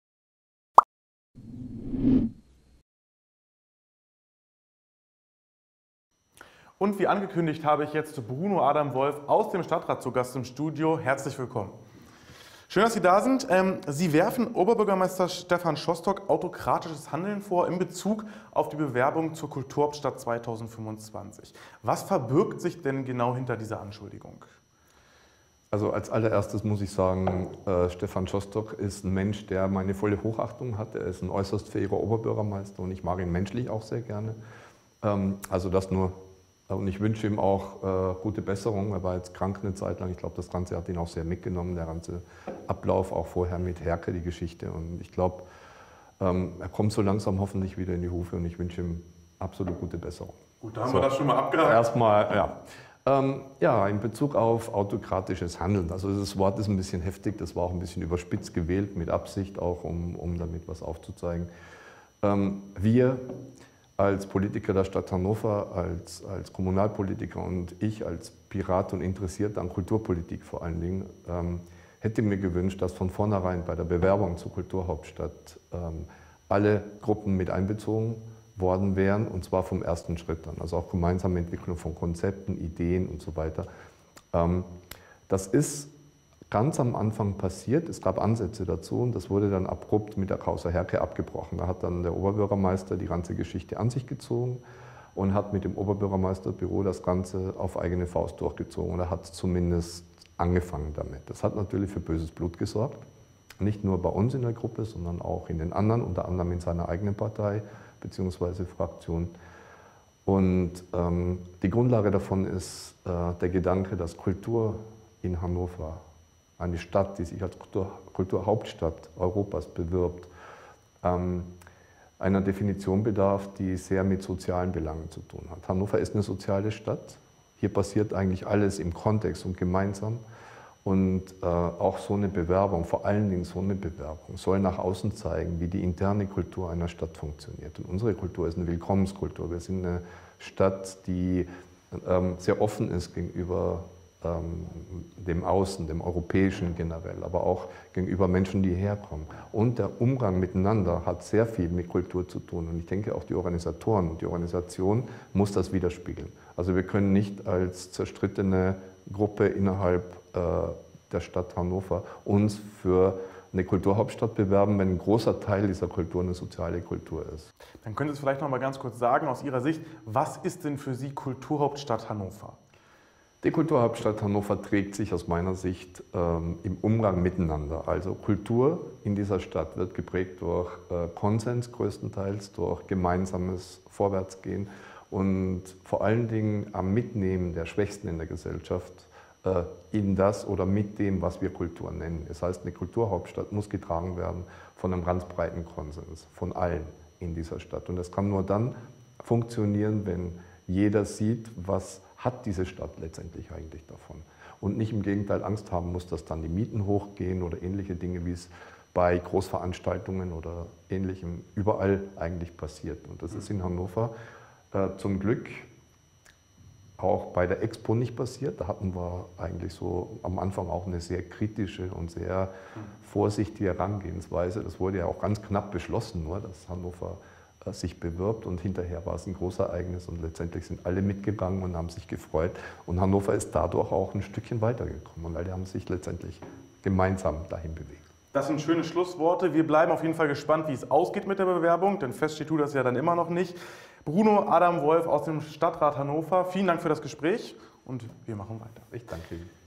Bruno Adam Wolf ist zu Gast bei 0511/tv.lokal
Bruno Adam Wolf, Die Piraten, ist zu Gast bei uns im Studio zum Thema ,,Autokratischer Stil von OB Schostok für Bewerbung als Kulturhauptstadt nicht förderlich"